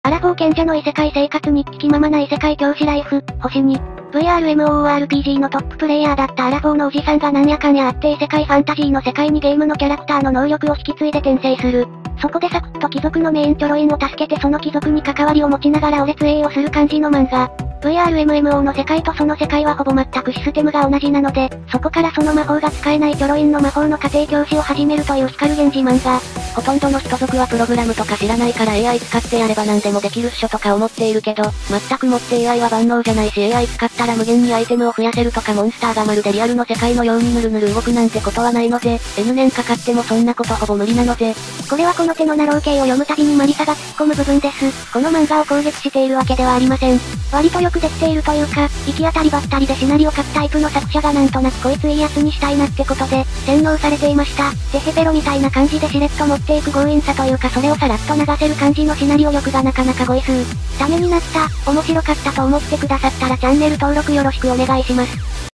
おすすめ漫画!! アラフォー賢者の異世界生活日記 ～気ままな異世界教師ライフ～ あらすじ / 寿安清 招來
アラフォー賢者の異世界生活日記 ～気ままな異世界教師ライフ～、☆２

VRMOORPGのトッププレイヤーだったアラフォーのOJSNがなんやかんやあって異世界ファンタジーの世界にゲームのキャラクターの能力を引き継いで転生する

そこでサクッと貴族のメインチョロインを助けてその貴族に関わりを持ちながら俺ツエエエをする感じの漫画

VRMMOの世界とその世界はほぼ全くシステムが同じなので、そこからその魔法が使えないチョロインの魔法の家庭教師を始めるという光源氏漫画

殆どの人族はプログラムとか知らないからAIつかってやればなんでもできるっしょとか思っているけど、全く持ってAIは万能じゃないしAI使ったら無限にアイテムを増やせるとかモンスターがまるでリアルの世界の用にぬるぬる動くなんてことは無いのぜ、N年かかってもそんなことほぼ無理なのぜ

これはこの手のなろう系を読むたびにまりさが突っ込む部分です、この漫画を攻撃しているわけではありません

割とよく出来ているというか、行き当たりばったりでシナリオ書くタイプの作者がなんとなくコイツいいやつにしたいなって事で、洗脳されていました、てへぺろみたいな感じでしれっと持って行く強引さというかそれをさらっと流せる感じのシナリオ力が中々ゴイスー